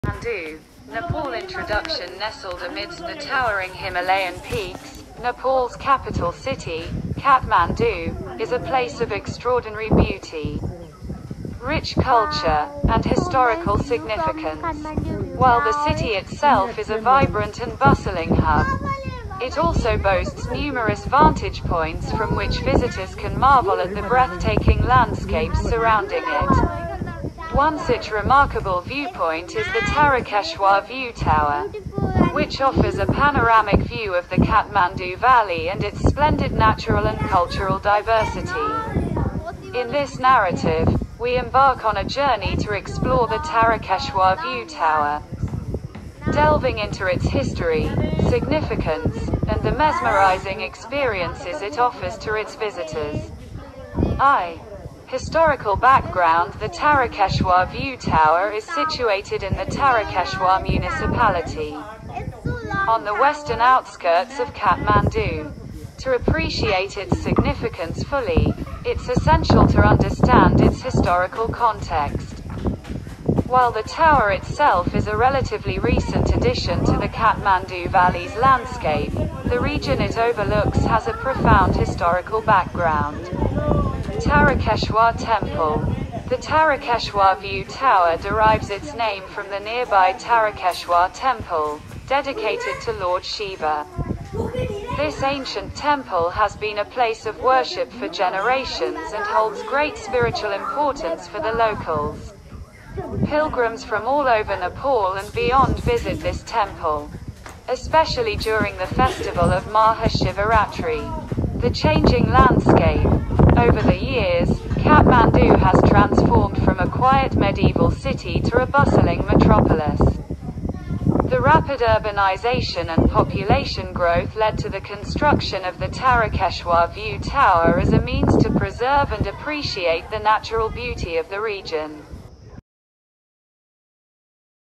Tarakeshwar view Tower Kathmandu || A Majestic Perspective of Kathmandu
The Tarakeshwar View Tower in Kathmandu, Nepal, offers a panoramic view of the Kathmandu Valley and its natural and cultural diversity. Named after the Tarakeshwar Temple, the tower is a recent addition to the region's landscape, reflecting the city's rapid urbanization and population growth. The tower's strategic location provides a 360-degree panorama of the Kathmandu Valley, offering a glimpse of the Himalayan mountain range, cultural landmarks like the Swayambhunath Stupa, Boudhanath Stupa, and Pashupatinath Temple, each a unique story of Nepal's history and spirituality. The tower also provides a glimpse into Kathmandu's urban landscape, with its maze of streets, markets, and colorful rooftops creating a dynamic contrast against the serene mountains. Beyond its cultural and architectural significance.